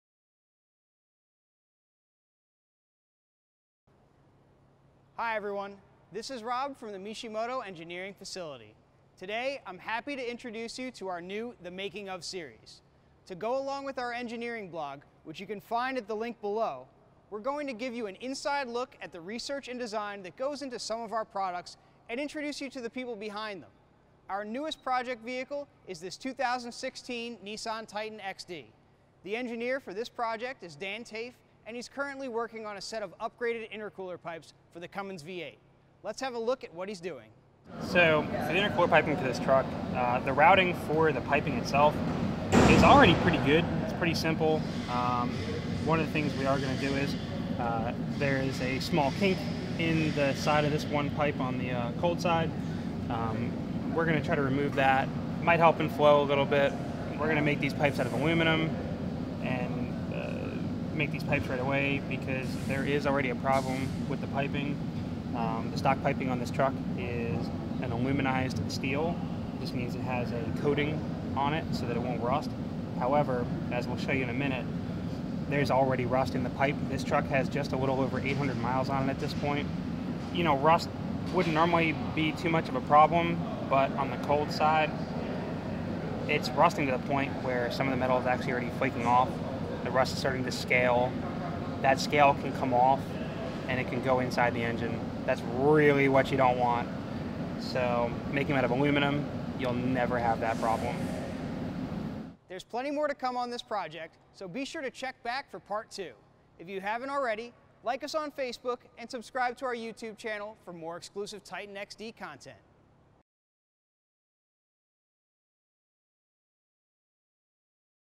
The Making Of: 2016-2019 Nissan Titan XD Intercooler Piping
Mishimoto's "The Making Of" series. This video features the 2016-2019 Nissan Titan XD Intercooler piping and how we plan to design our own upgraded version. Follow our progress at engineering.mishimoto.com